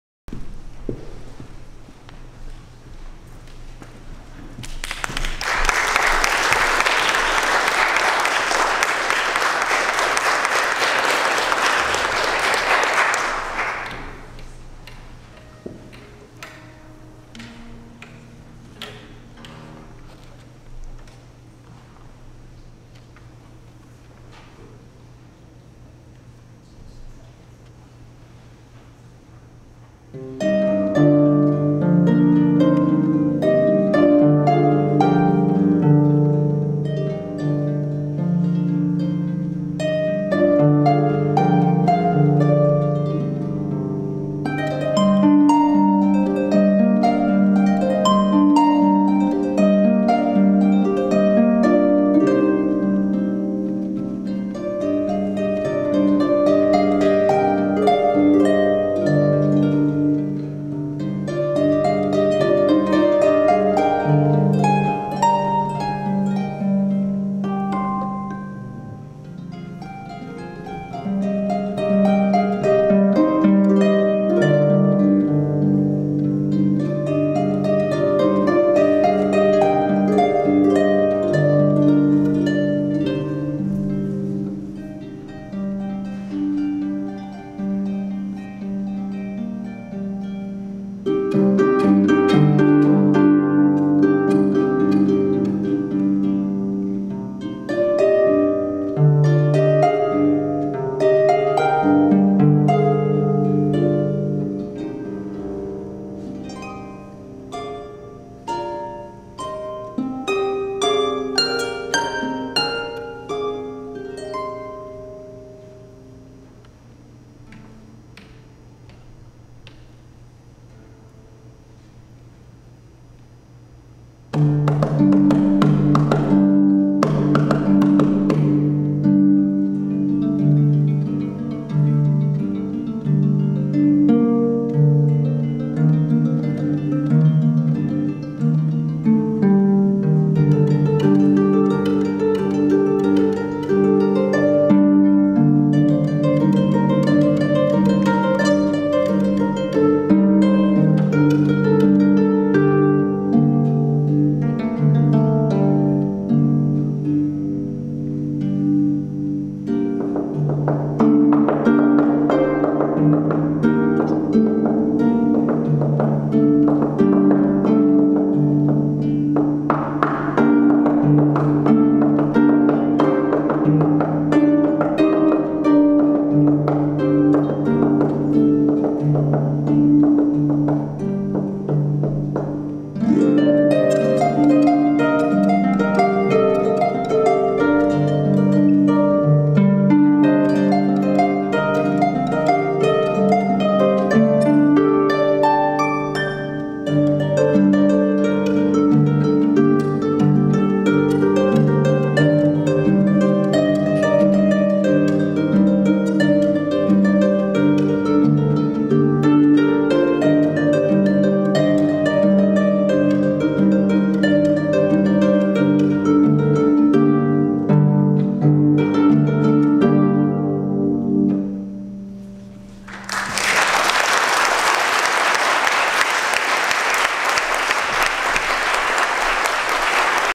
Category I Winner 2019 Nathan Zhou
The American Harp Society in Dallas is pleased to announce the winners of the 2019 Student Scholarship Award Competition and the inaugural Gail Barber Scholarship Prize Competition.  The competitions were held on Saturday, April 6, 2019 at the Northeast Campus of Tarrant County College.